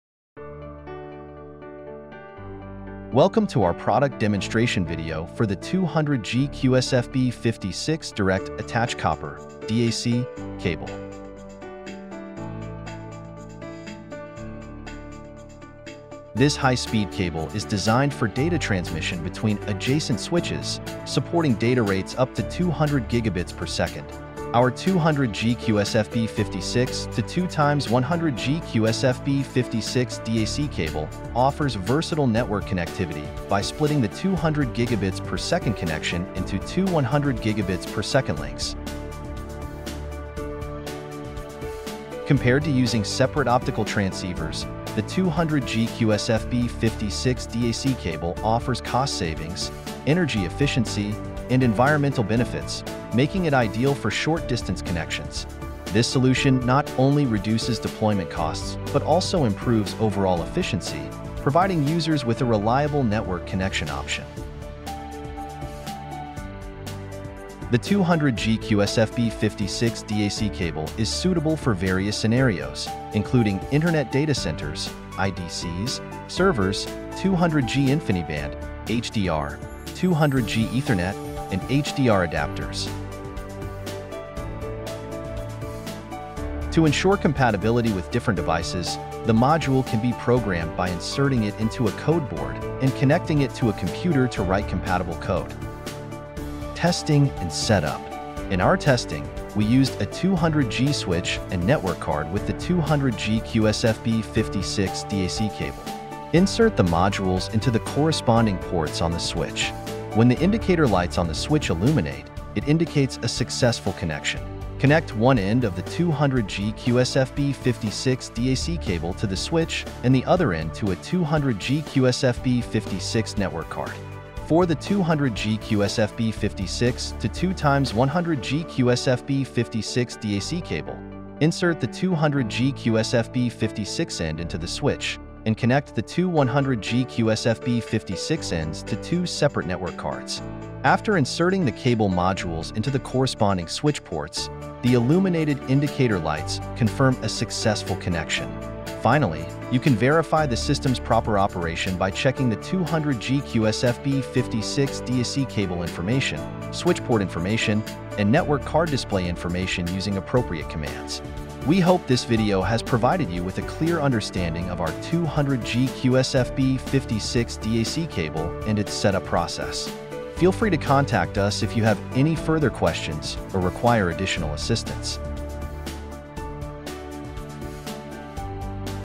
200G QSFP56 DAC Introduction and Application | FiberMall
200G QSFP56 to 2x 100G QSFP56 DAC cable offers versatile network connectivity by splitting the 200Gb/s connection into two 100Gb/s links.
Compared to using separate optical transceivers, the 200G QSFP56 DAC cable offers cost savings, energy efficiency, and environmental benefits, making it ideal for short-distance connections. This solution reduces deployment costs and improves overall efficiency, providing users with a reliable network connection option.